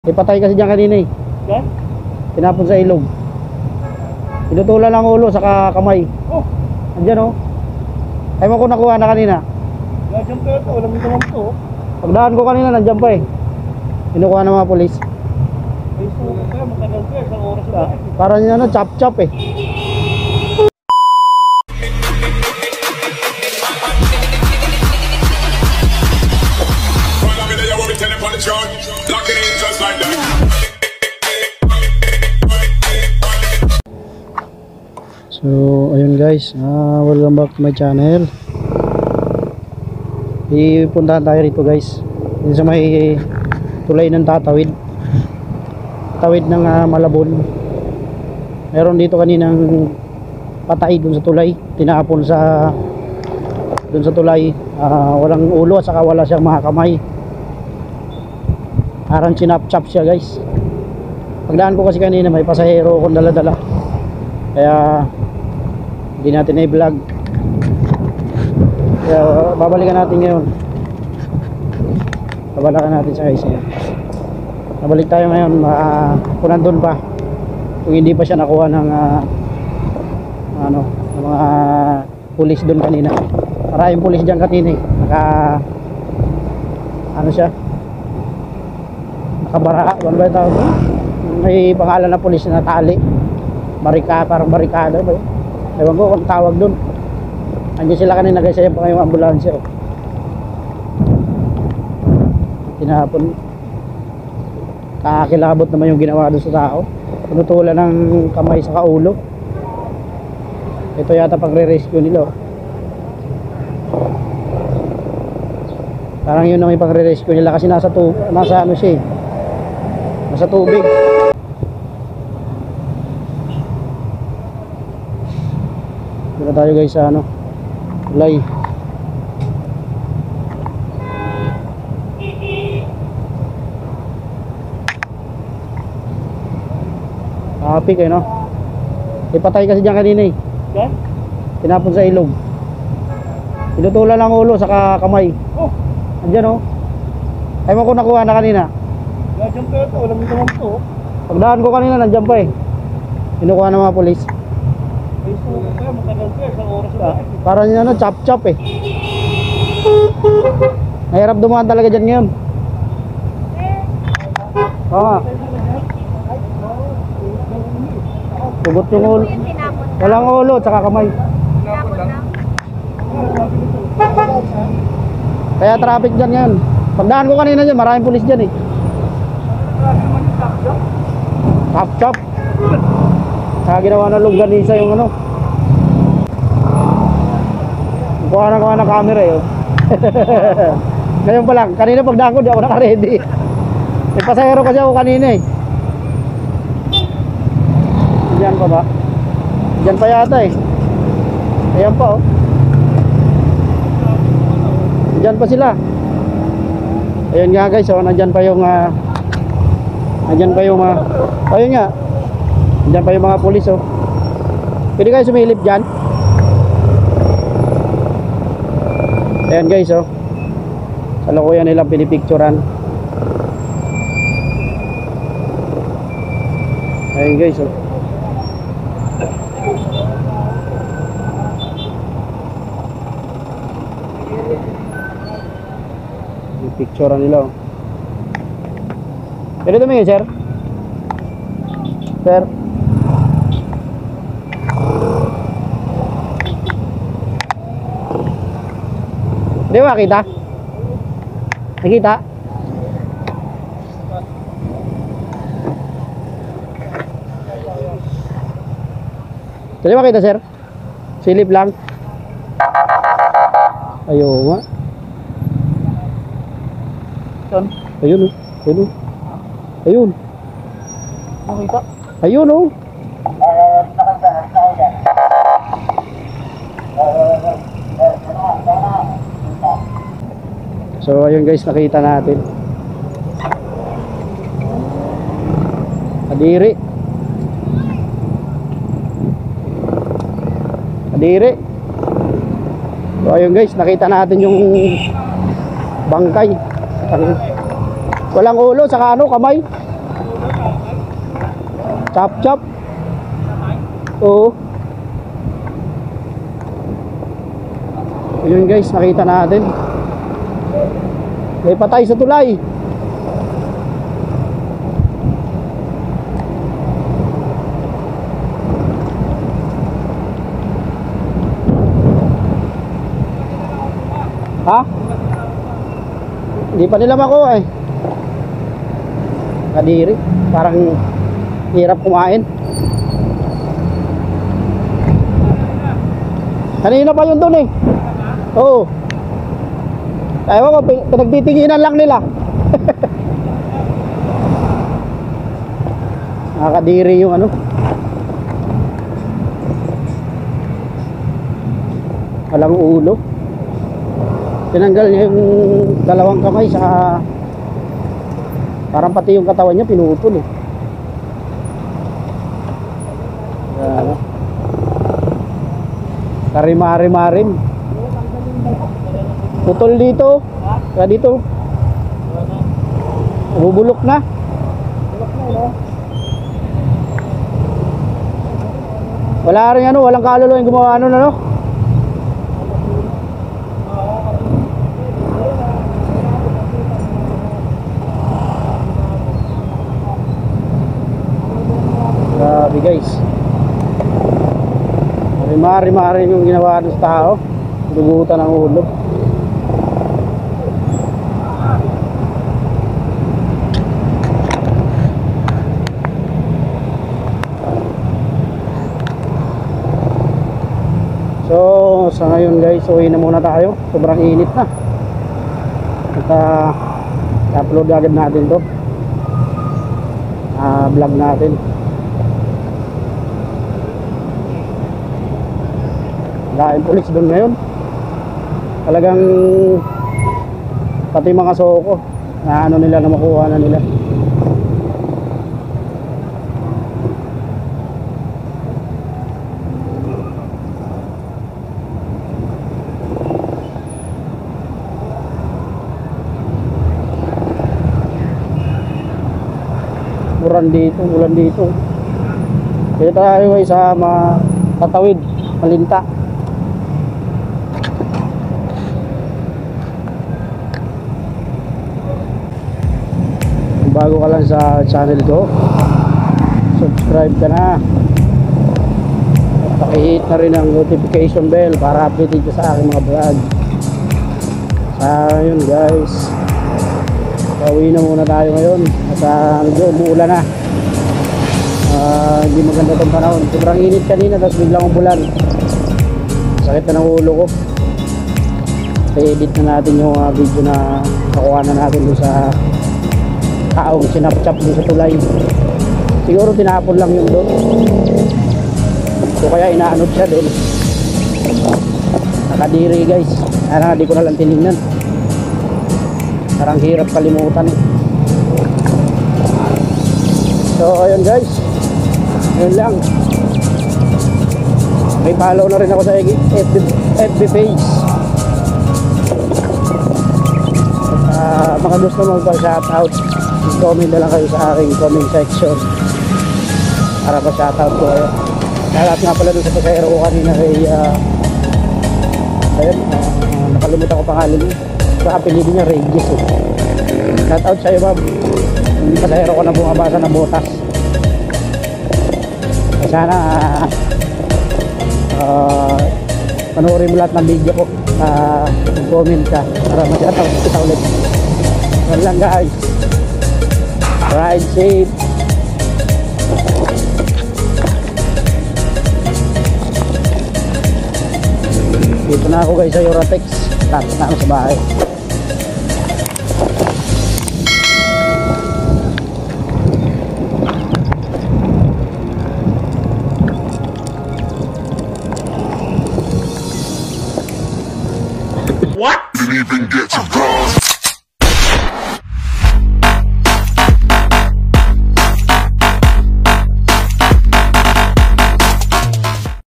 CHOP CHOP VICTIM TINAPON SA TULAY NG TATAWID VALENZUELA KARUMAL DUMAL SINAPIT NG BIKTIMA
CHOP CHOP VICTIM TINAPON SA TULAY NG TATAWID VALENZUELA KARUMAL DUMAL SINAPIT NG BIKTIMA WALANG ULO AT MGA BRASO